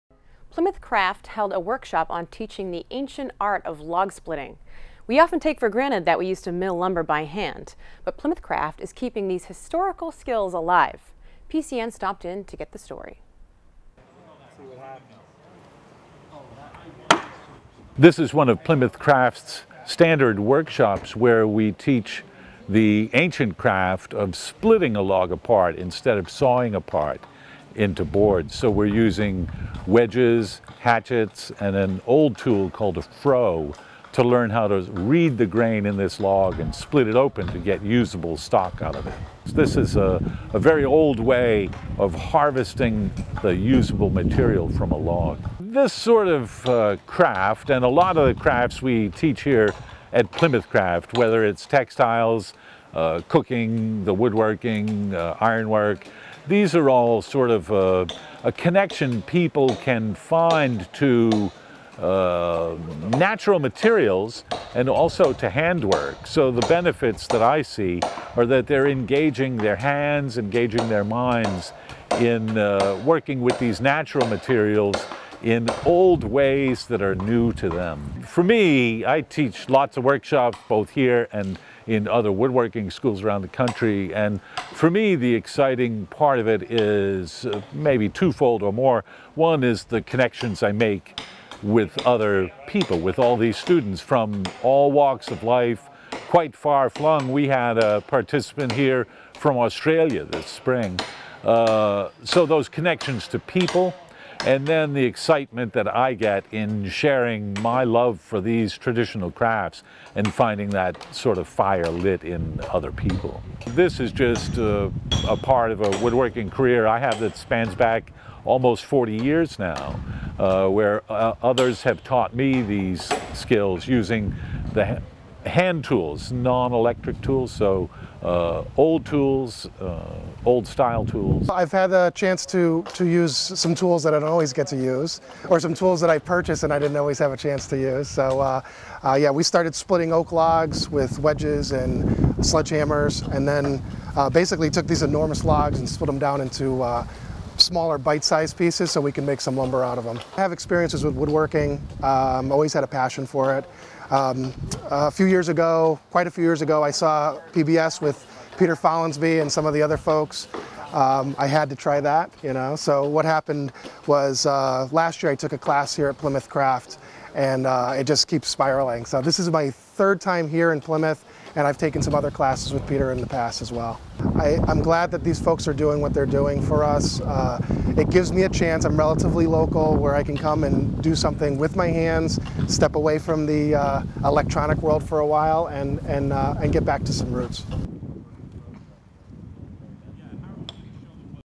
Plymouth CRAFT Riving and Hurdle making Classes
PCN PACTV Community News- November 16th, 2017

Plymouth MA- Plymouth craft held a workshop on teaching the ancient craft of log splitting. We often take for granted that we used to mill lumber by hand but Plymouth Craft is keeping these historical skills alive. PCN stopped in to get the story!